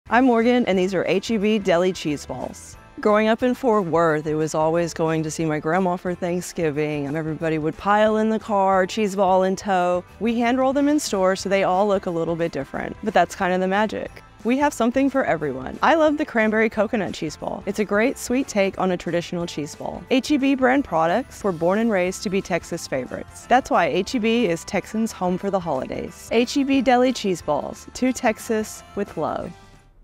H-E-B Deli Cheeseballs | To Texas, With Love
H-E-B brand products were born and raised to be Texas favorites, that’s why H-E-B is Texans’ home for the holidays. H-E-B Deli cheeseballs - to Texas, with love.